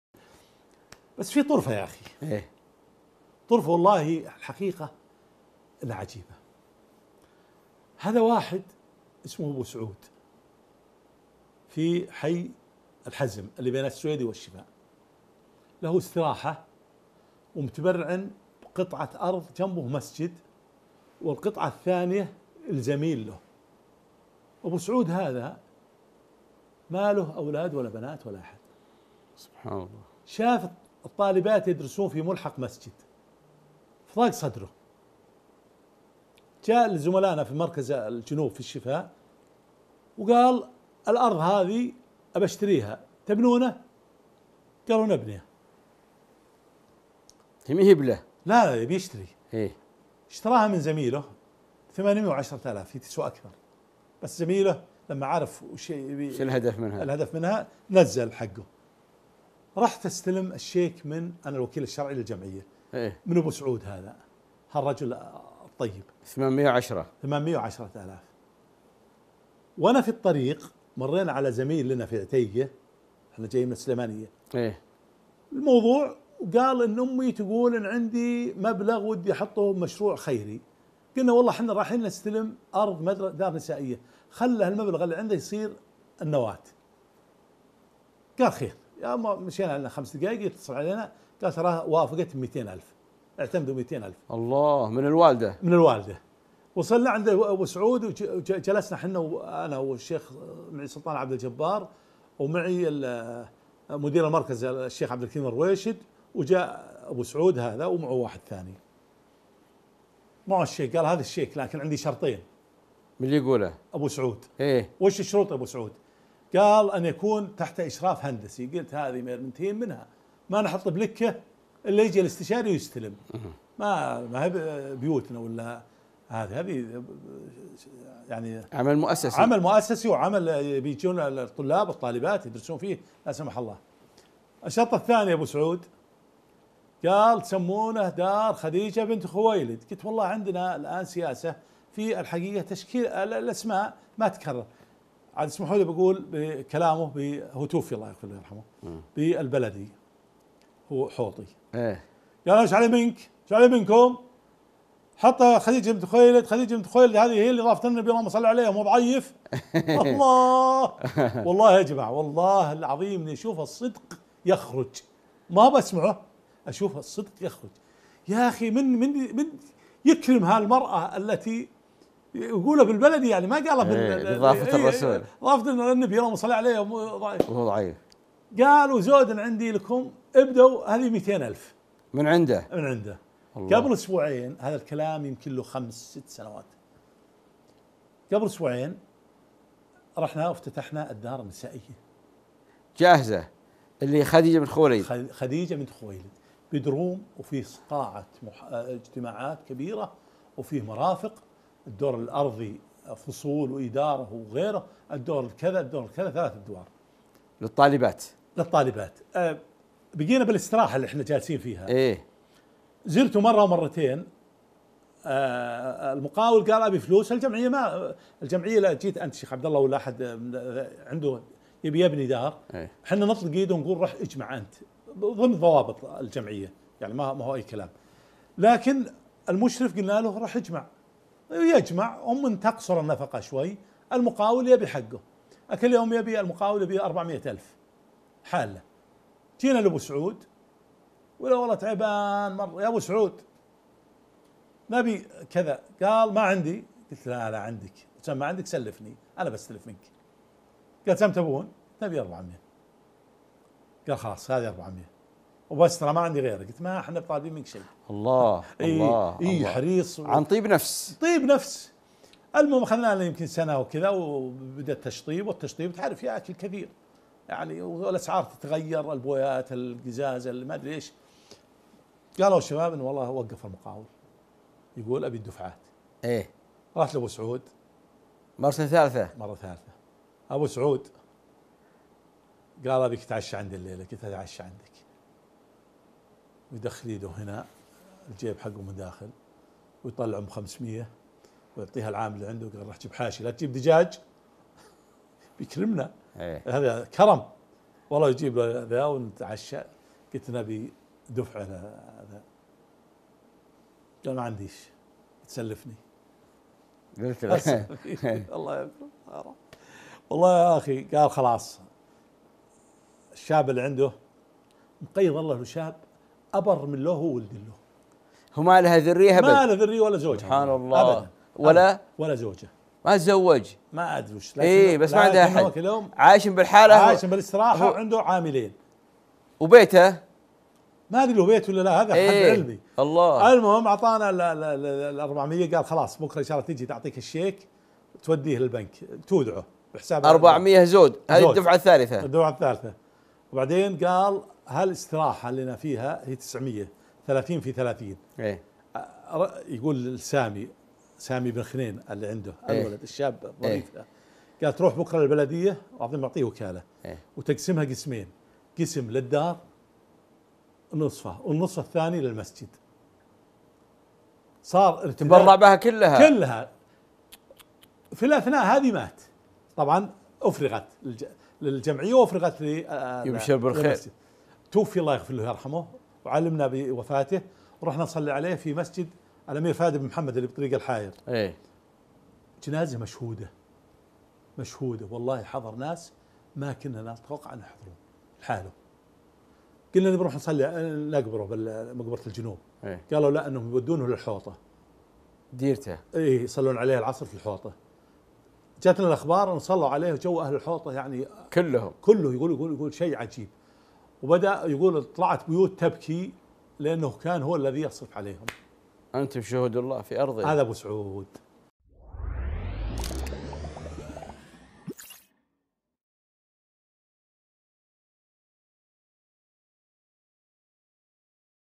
رجل لم يتزوج شاف طالبات التحفيظ يدرسون في ملحق المسجد فتبرع لهم بدار خاصة لهم لكن بشرطين!
يعرض كل جمعة
بعد المغرب من مكة المكرمة
من تقديم :
عبدالله بن باز
على
ضيف الحلقة:
 فضيلة الشيخ/عبدالرحمن بن عبدالله الهذلول

13 قناة آمنة لأسرتك!